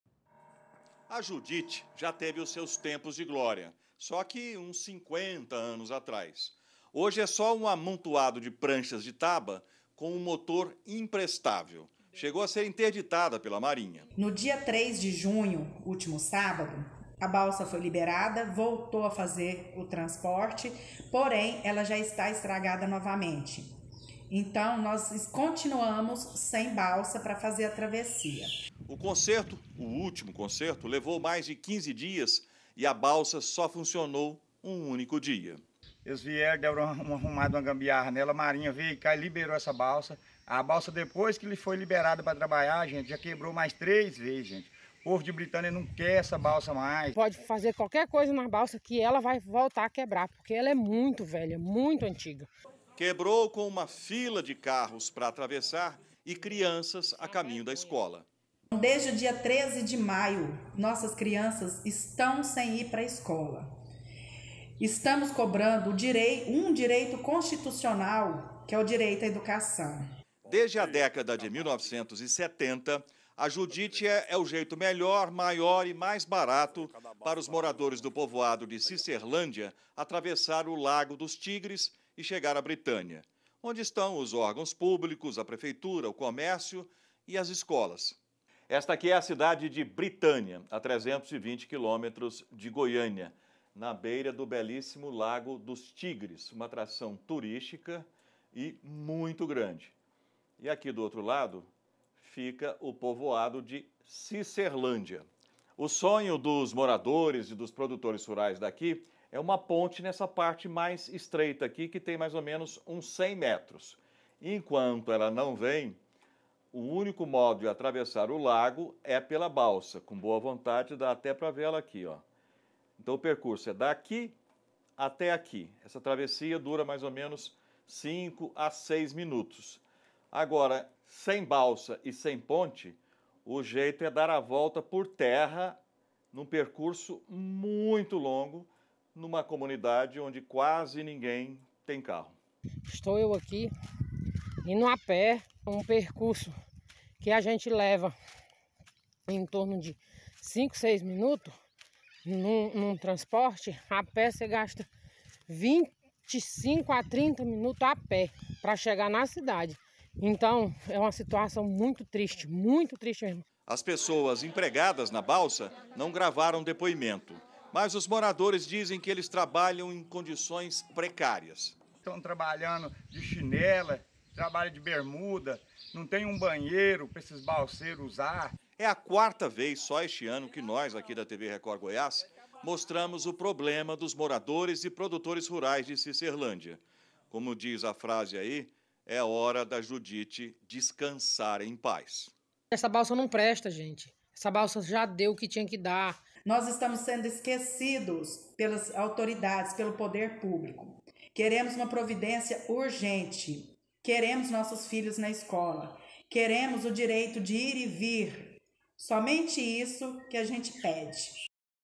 DE NOVO: BALSA QUEBRA PELA QUARTA VEZ E DEIXA MORADORES ILHADOS
A JUDITE ESTÁ DE VOLTA. DE VOLTA AO NOTICIÁRIO PORQUE QUEBROU DE NOVO. JUDITE É O NOME DE UMA VELHA BALSA QUE FAZ A TRAVESSIA DE MORADORES E PRODUTORES RURAIS DO DISTRITO DE CICERLÂNDIA E BRITÂNIA, SEDE DO MUNICÍPIO. O PESSOAL QUE USA A JUDITE SONHA COM UMA SOLUÇÃO MAIS DEFINITIVA, UMA PONTE. MAS JÁ FICARIAM SATISFEITOS COM UMA BALSA NOVA. PORQUE A VELHA, NÃO DÁ MAIS.